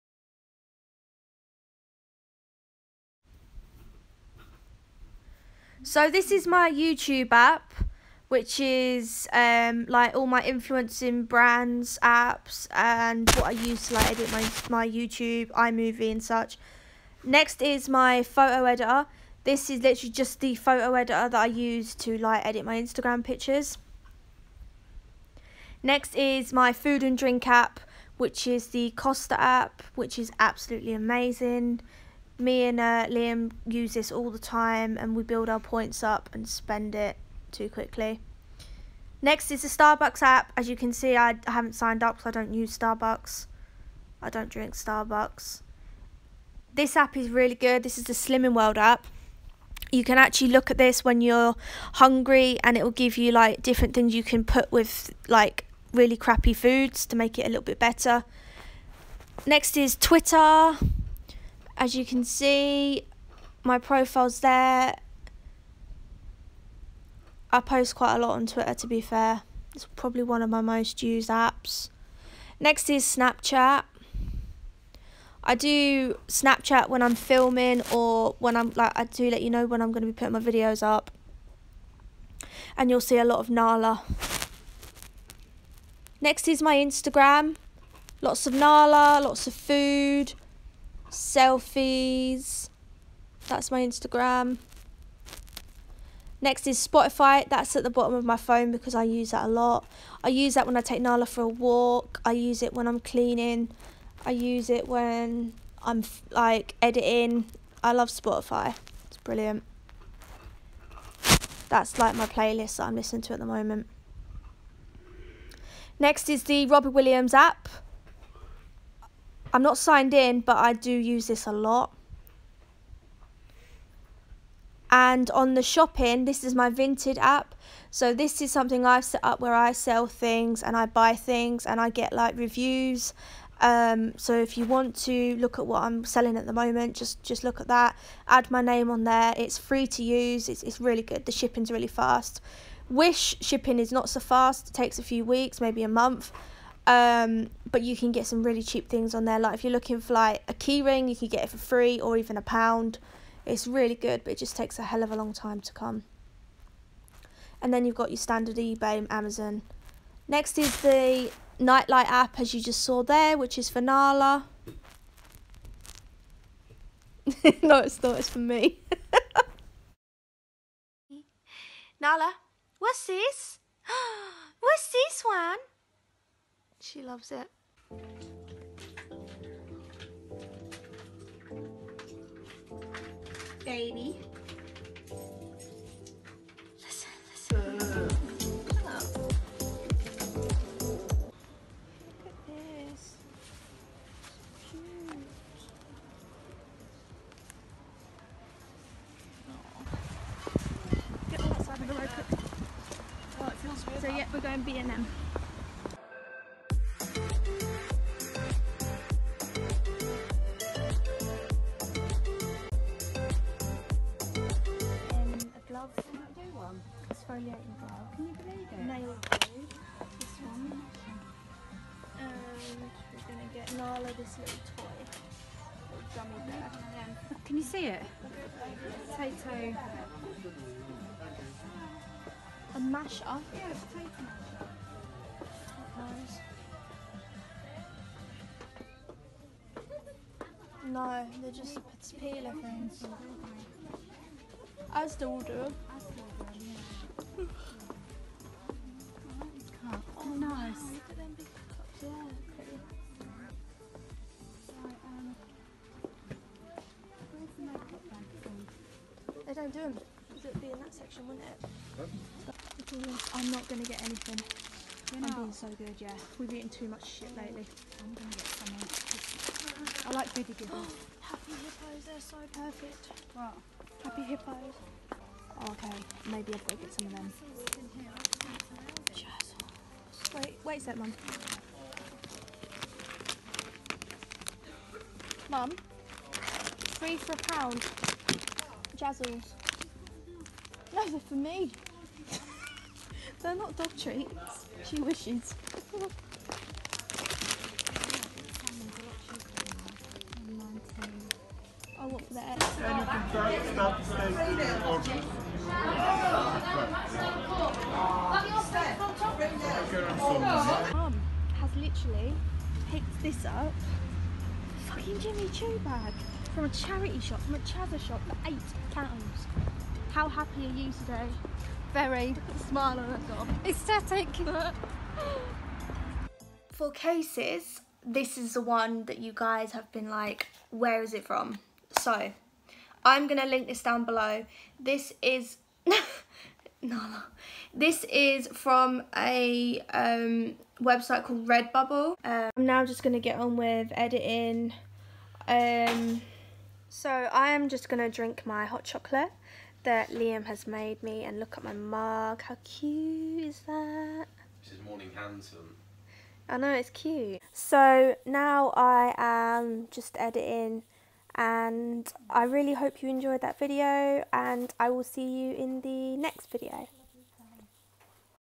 WHATS ON MY IPHONE
Watch this video to see what’s on my iPhone 7 Plus and the case I use, also follow me around.